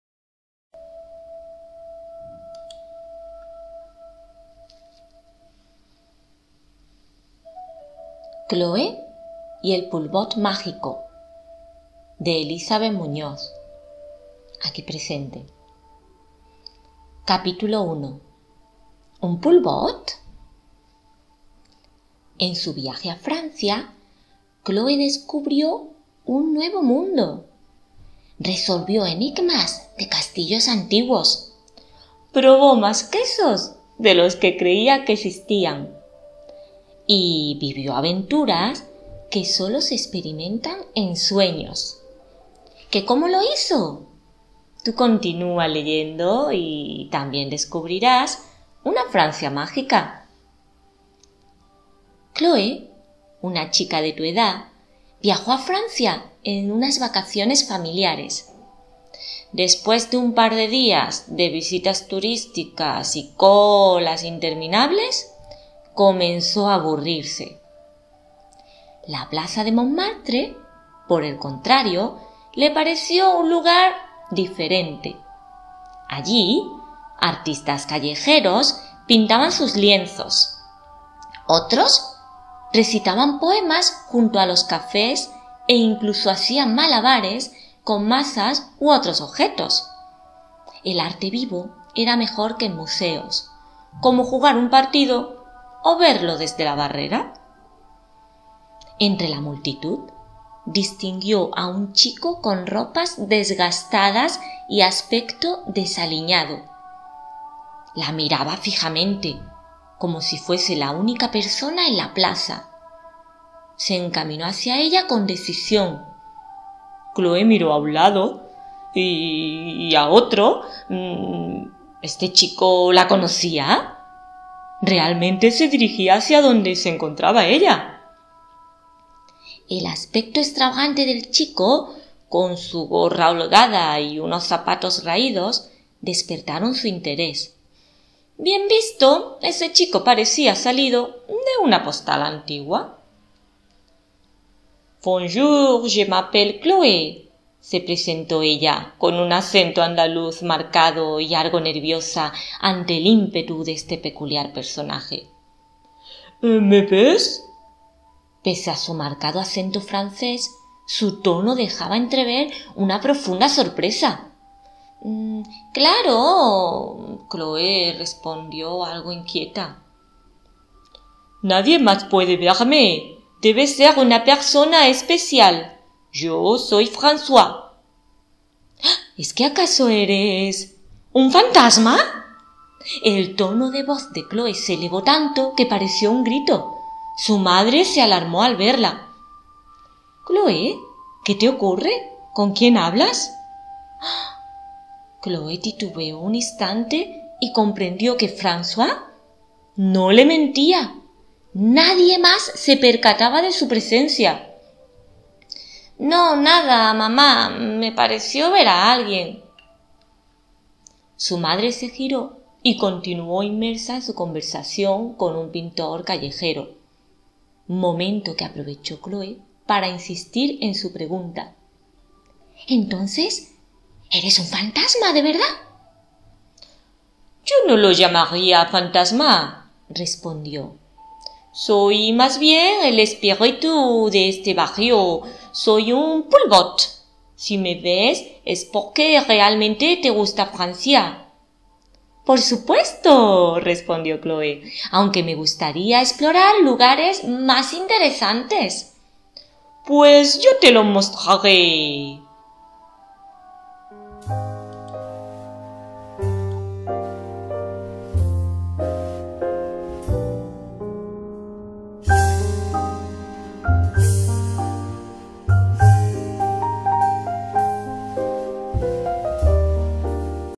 1. Cloe y el poubolt mágico por Elisabeth Muñoz Sánchez
Elisabeth, profesora y escritora de cuentos infantiles, nos envía el audio, leído por ella misma, del capítulo 1 de su libro Cloe y el poubolt mágico.
Feliz día del libro.